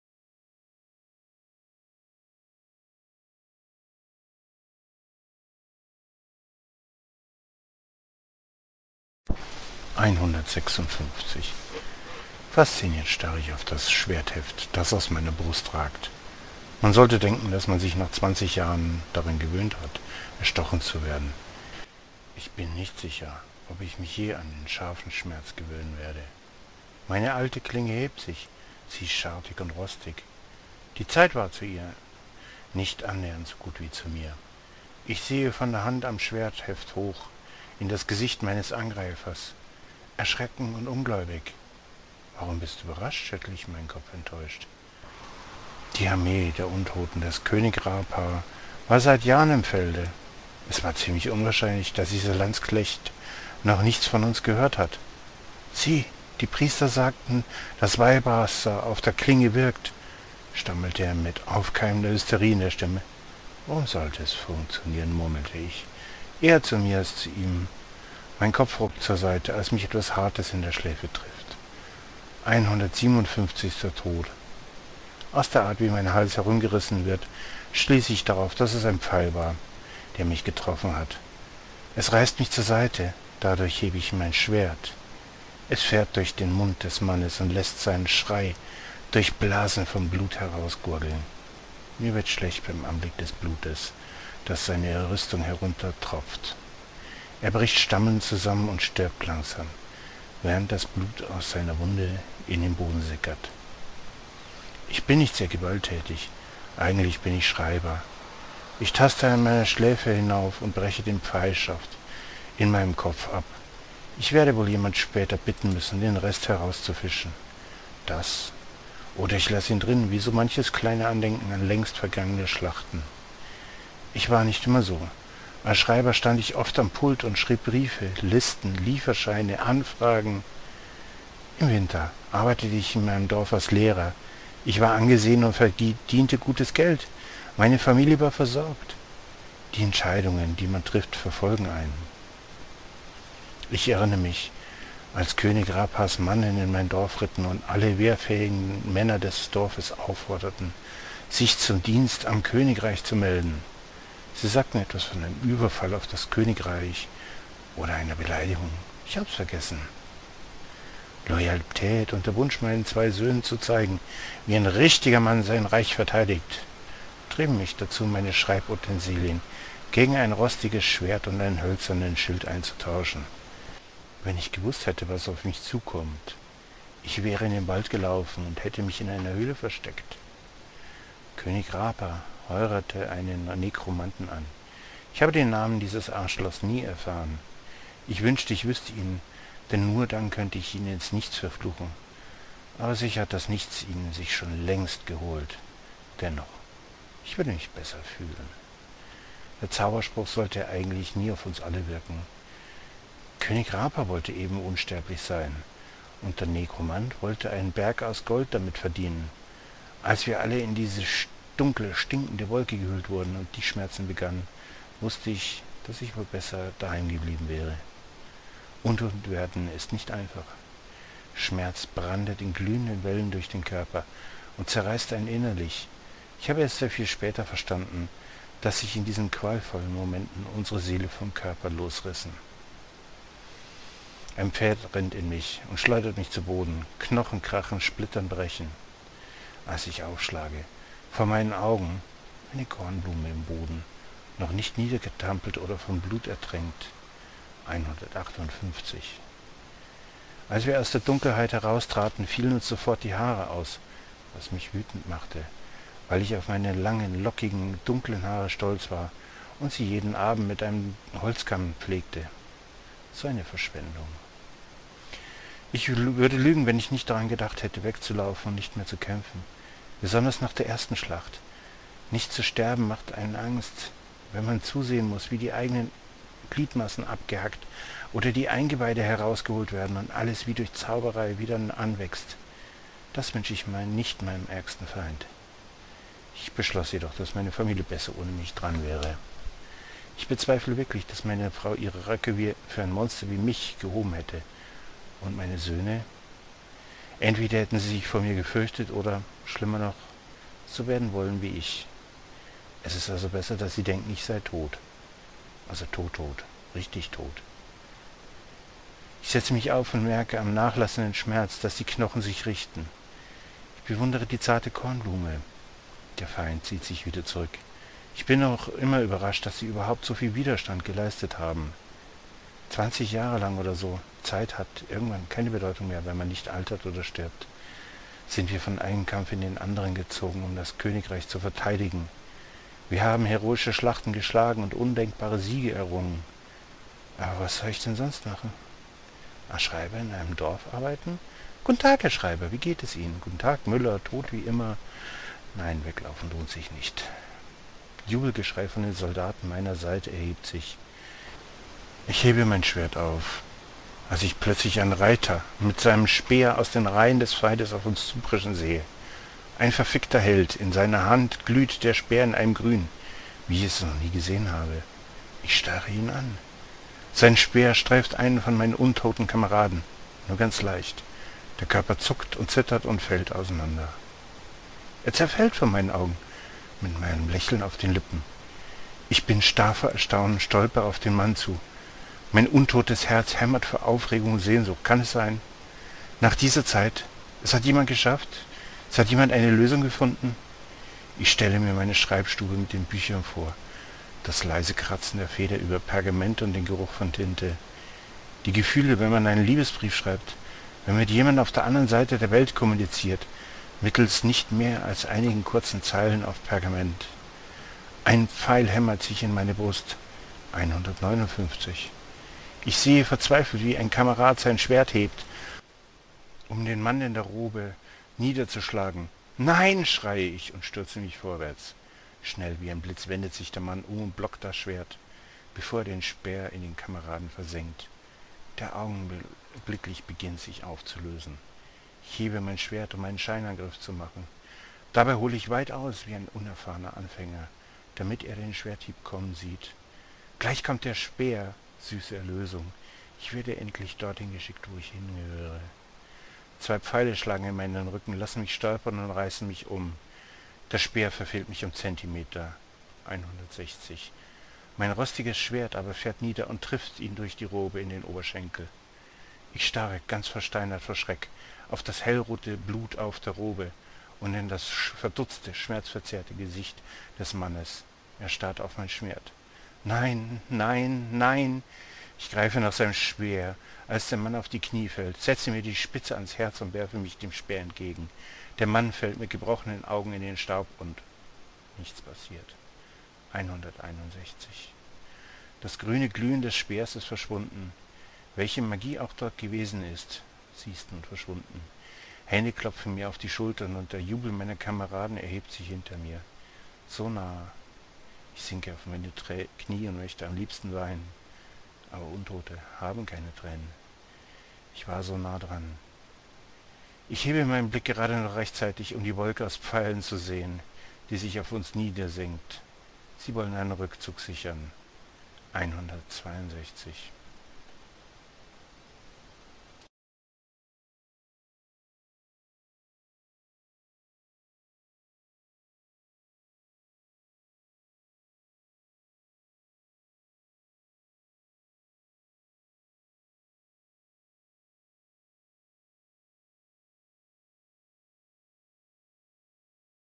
Einhundertsechsunfünfzig
Einhundertsechsundfünfzig.

Fasziniert starre ich auf das Schwertheft, das aus meiner Brust ragt. Man sollte denken, das man sich nach 20 Jahren daran gewöhnt hat, erstochen zu werden. Ich bin nicht sicher, ob ich mich je an den scharfen Schmerz gewöhnen werde.

(Sorry die schlechte Soundqualität )